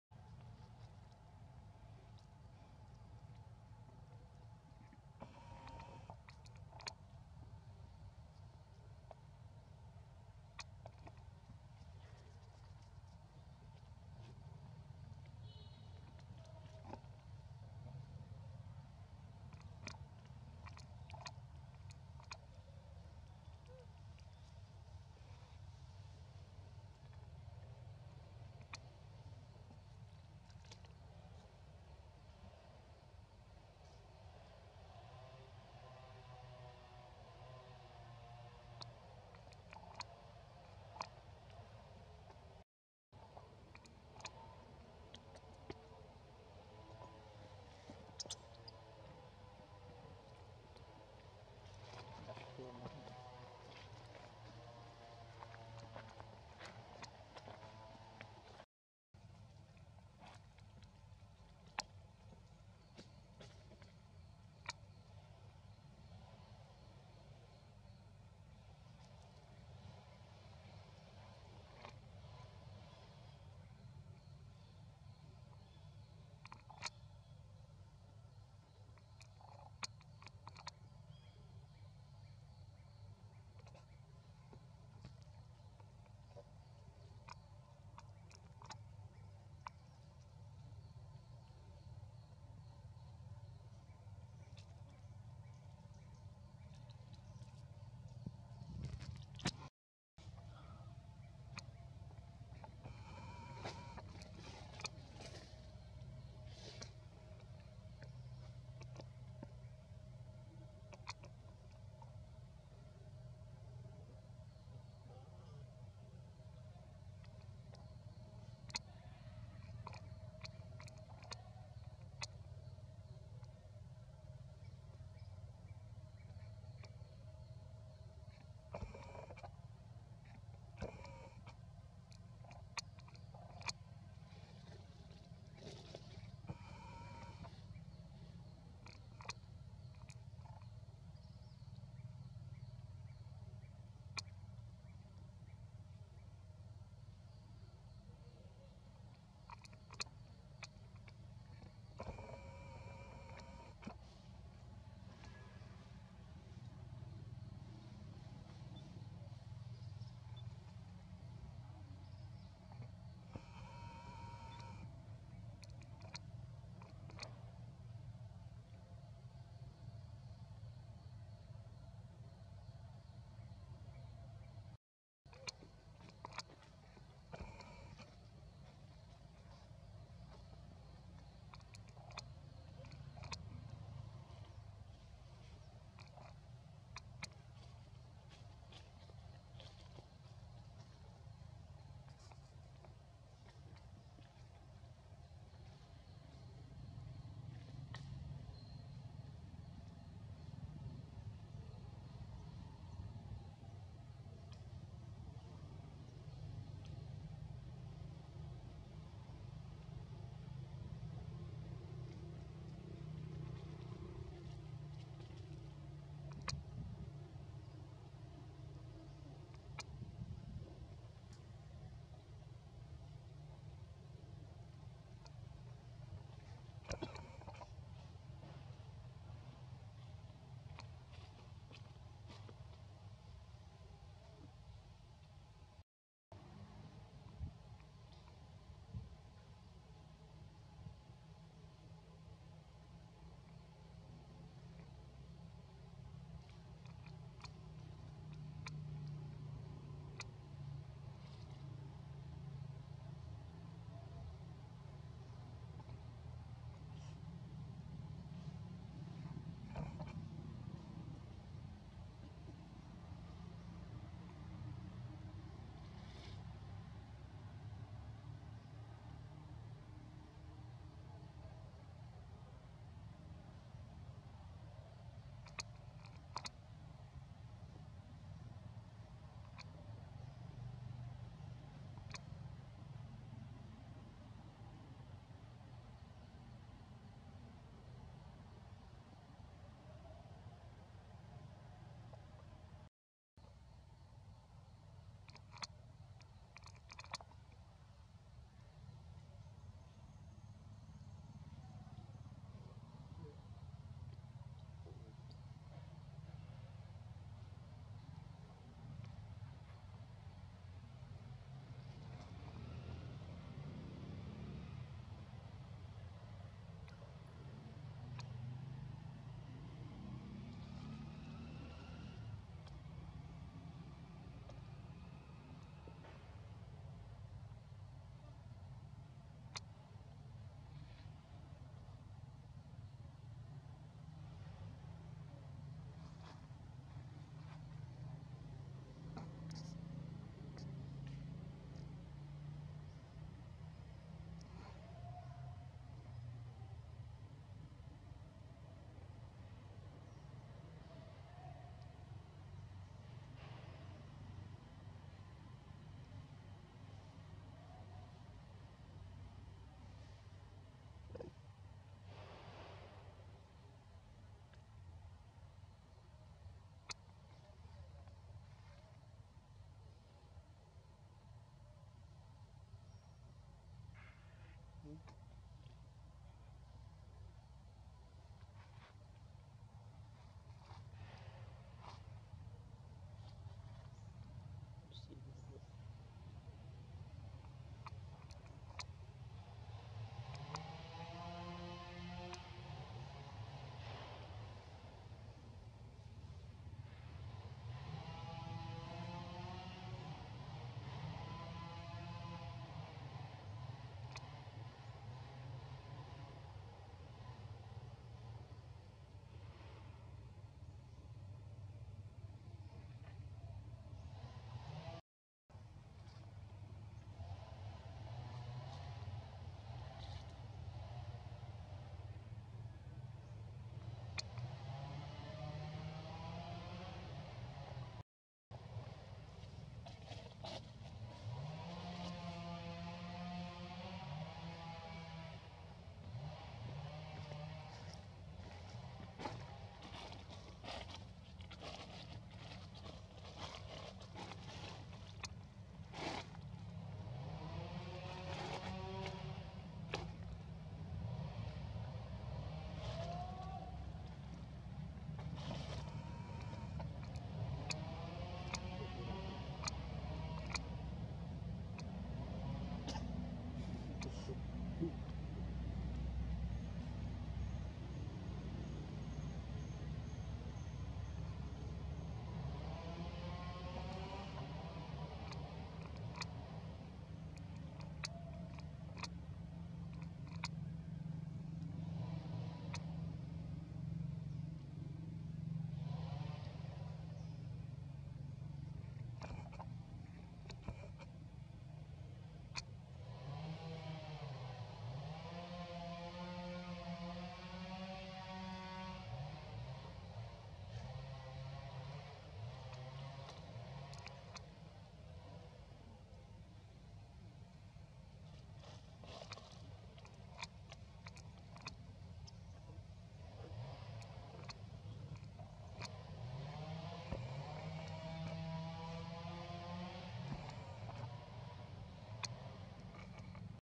Cutie GiGi Loves Her Milk Very Much And She Loves Playing With Her Friends | Their Moms Look After
Welcome to channel: @realmonkey2023
Cutie GiGi Loves Her Milk Very Much And She Loves Playing With Her Friends .
Other Reaction!
You can watch it even when you aren't connected.